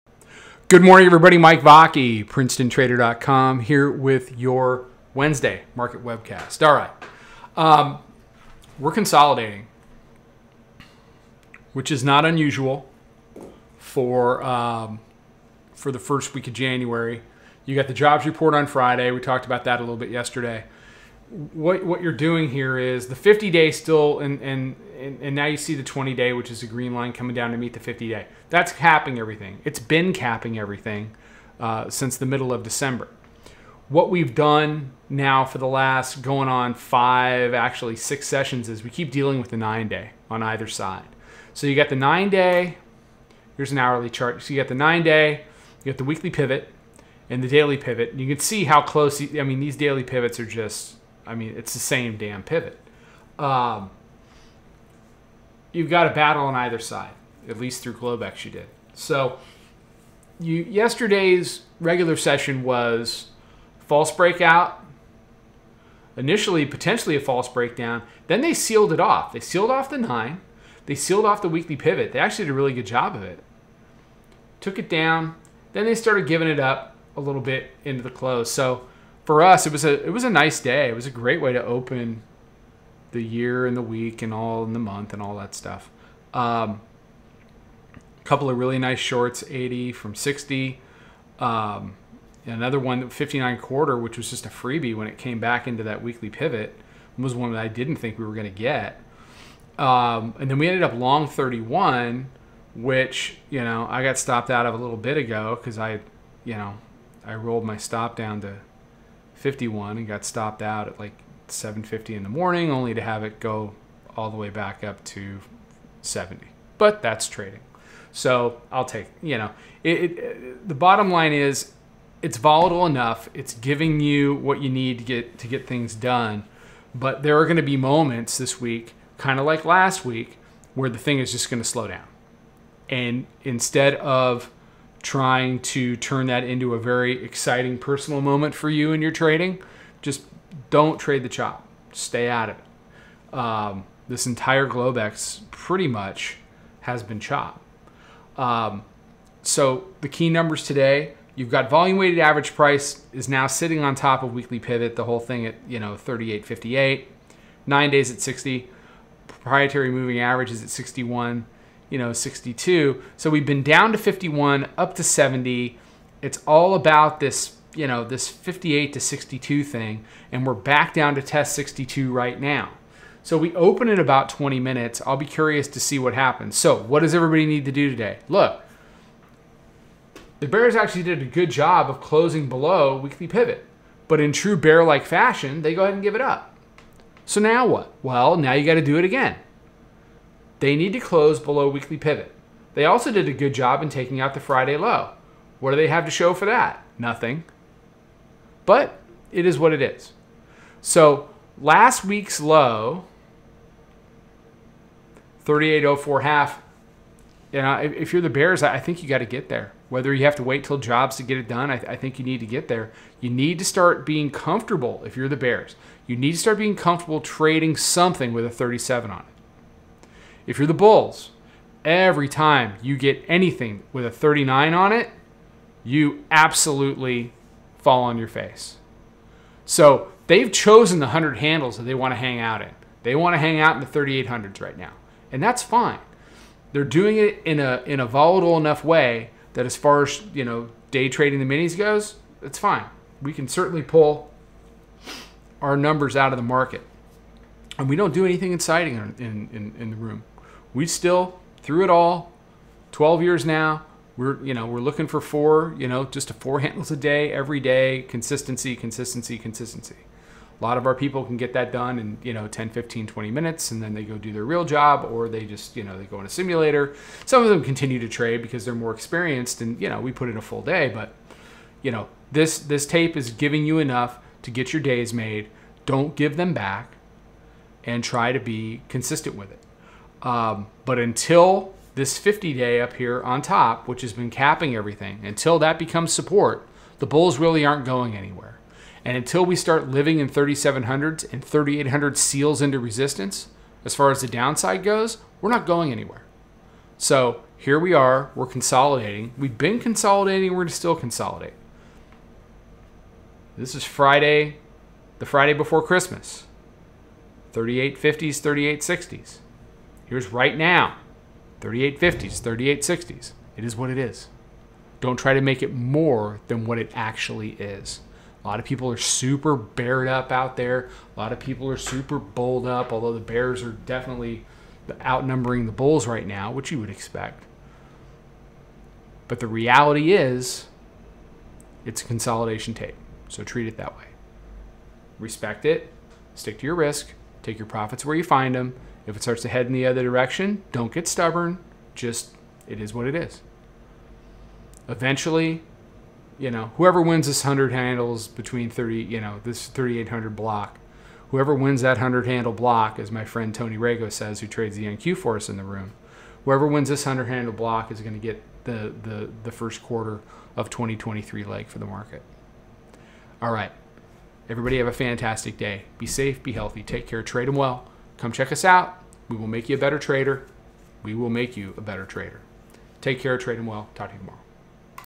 S&P Futures Webcast January 4, 2023 – How to Trade this Range
Mike Vacchi from Princetontrader Futures Trading Education in his Mid-Day Webcast analysis of S&P Futures for beginners and experienced traders seeking the best in Futures Trading Education. Risk focused trading for all!